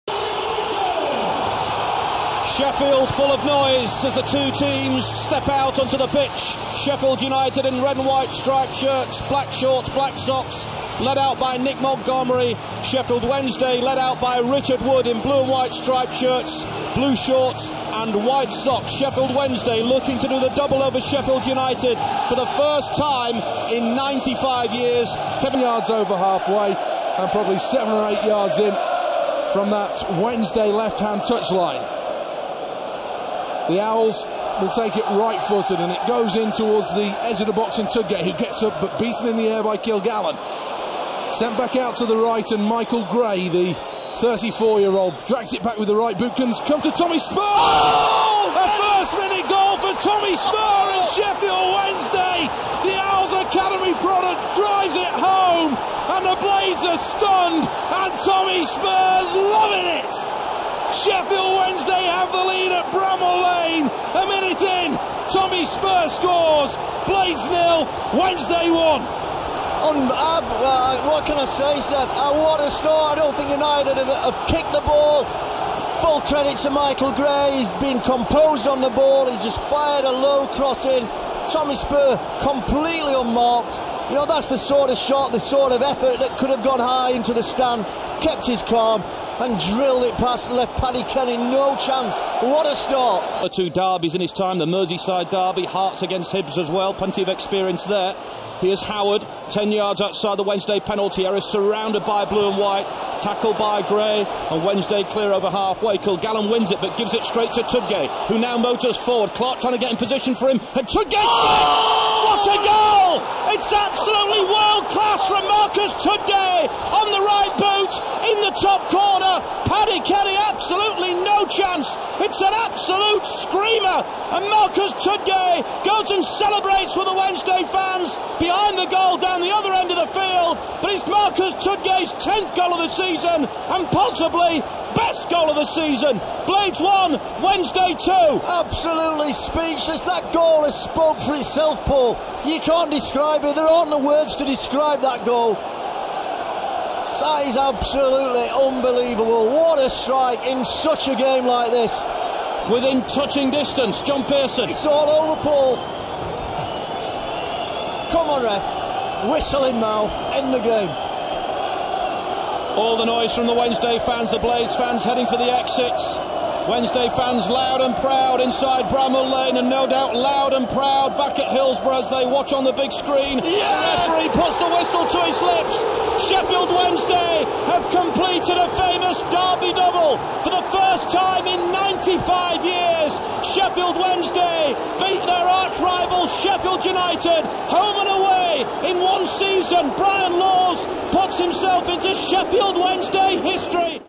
Derby double! | Sheffield United 1 Sheffield Wednesday 2 | 7 February 2009
Sheffield Wednesday defeated Sheffield United both home and away in the 2008/09 campaign. The derby double was confirmed after a 2-1 win over the Blades at Bramall Lane on 7 February 2009, under then manager Brian Laws.

Watch back and enjoy!